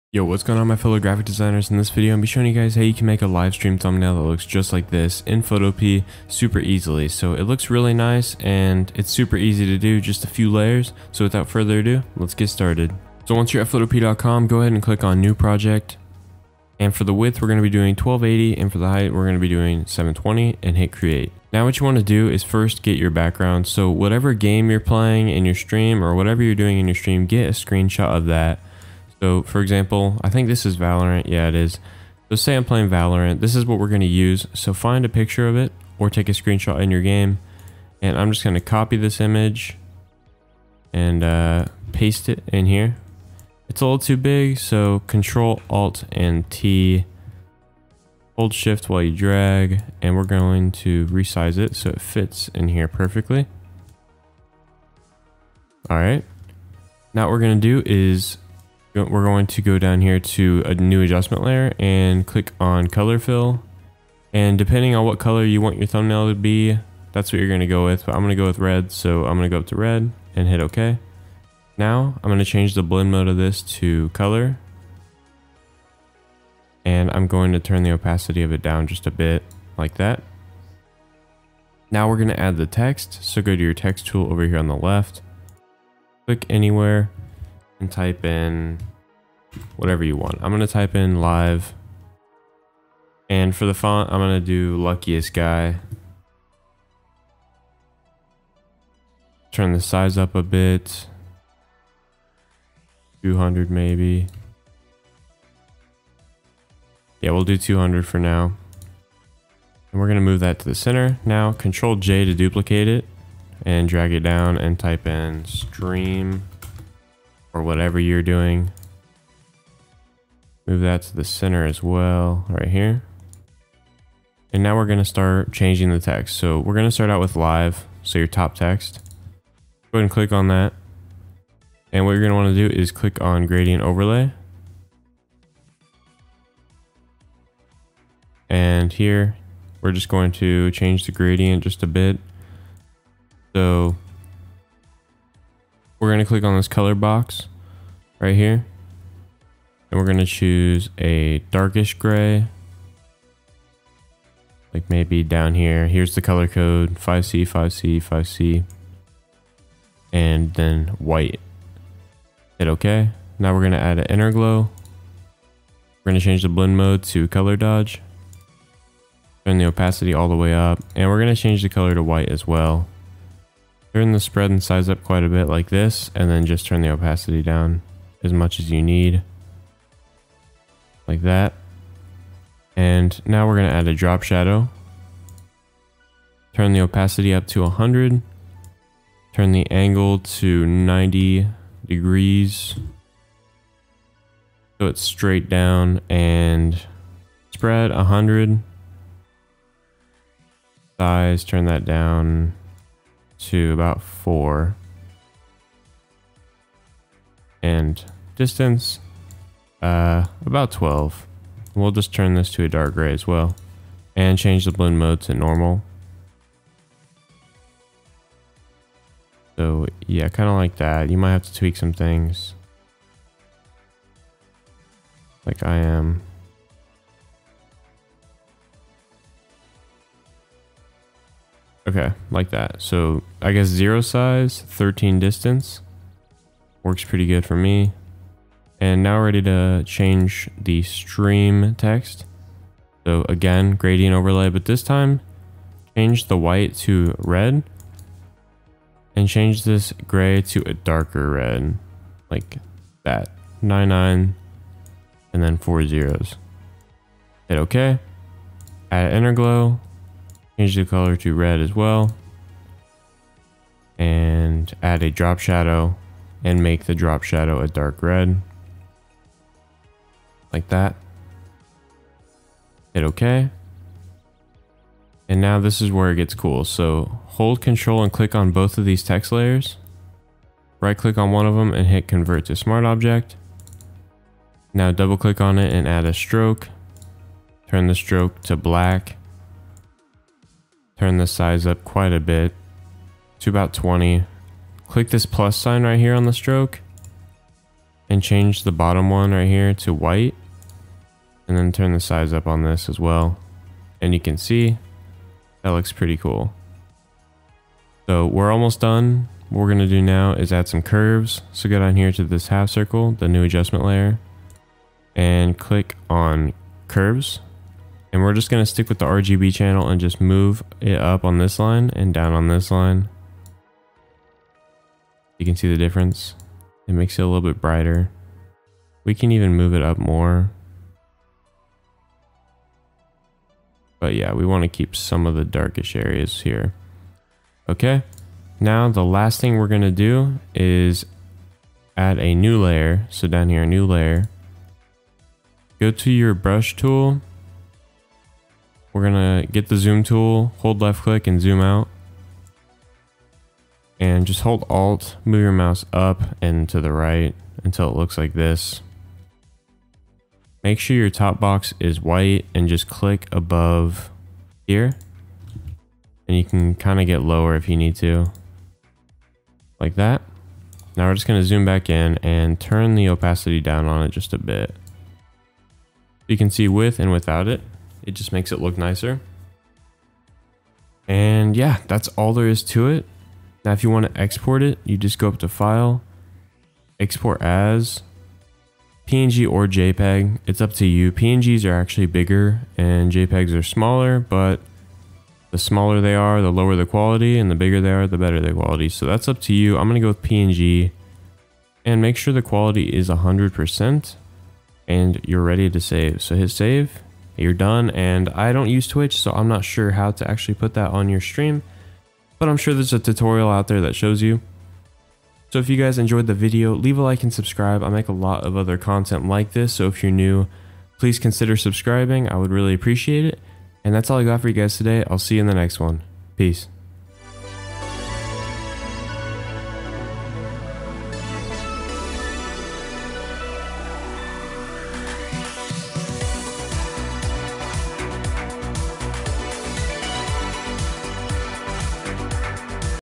How to make a LIVESTREAM THUMBNAIL for FREE *NO PHOTOSHOP*
In this video I show you how to make a livestream thumbnail for free without Photoshop! By the end of this video you should be able to make a custom thumbnail for your streams with ease!

► Resources
Photopea.com